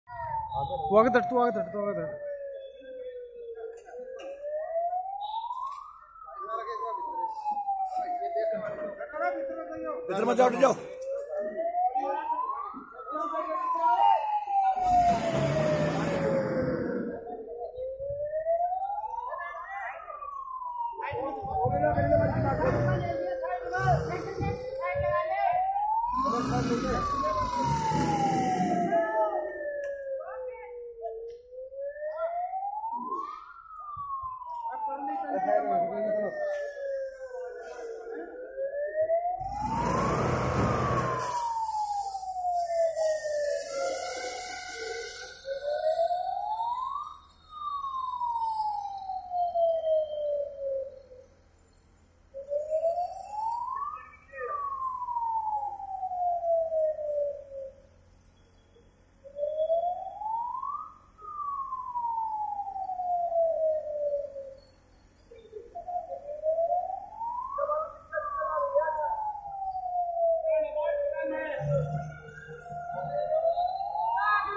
भिवानी -दादरी जिले की सीमा से स्टीक कस्बे झोझू के पावर हाउस में लगी भीषण आग-.. bhataia ji foundation
o=U&video_id=7ifWFkwB80o